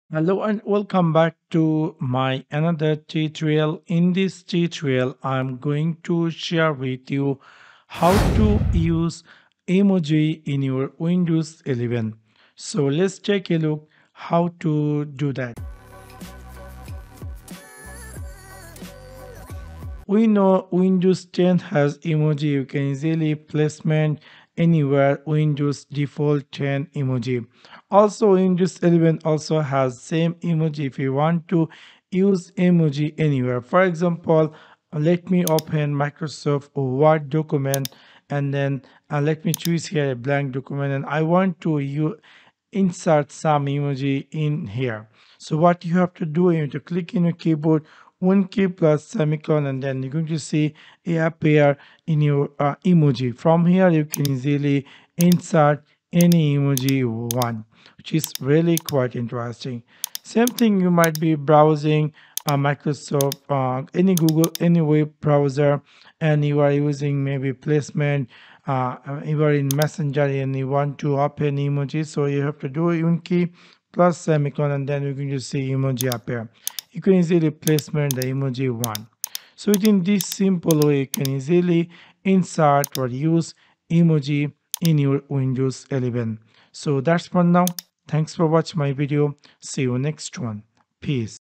😊 How to Use Emojis on Windows 11
Learn how to use emojis on Windows 11

You can do this by pressing the Windows and semicolon key on the keyboard. Another way you can do this is pressing the Windows key and the period key.

Now it is important to keep in mind that some apps don't support Windows 11's modern emojis. However, the classic emojis should work fine.